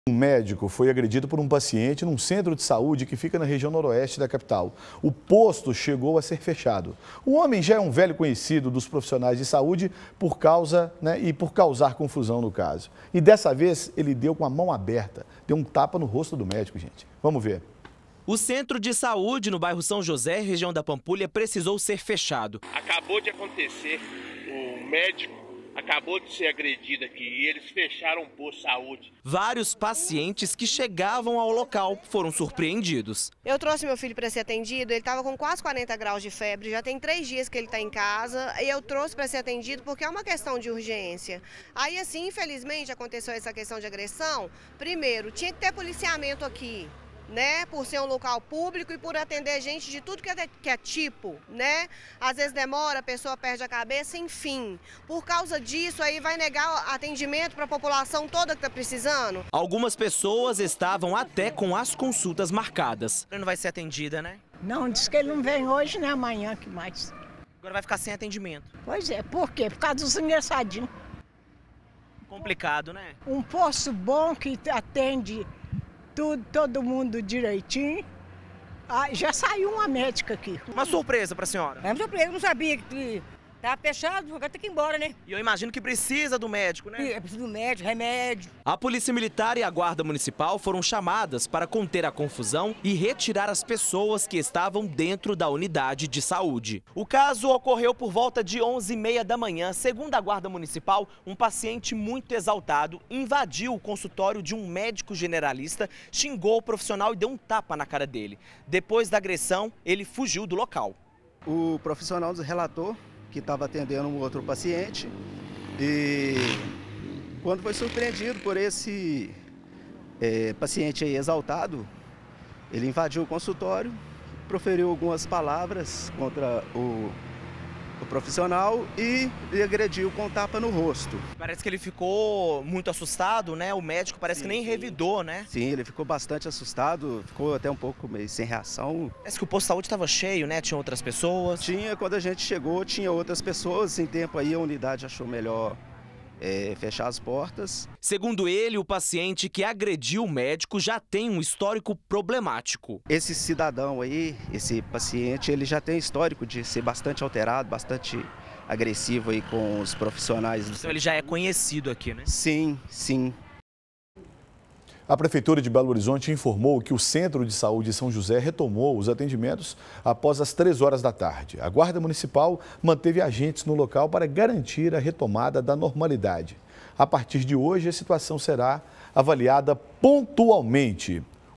Posto de saúde fica fechado após paciente dar tapa no rosto de um médico
Um médico foi agredido por um paciente em um centro de saúde, que fica na região noroeste de Belo Horizonte. O posto chegou a ser fechado. O homem já é um velho conhecido dos profissionais de saúde, por causar confusão. E, dessa vez, deu um tapa no rosto do médico.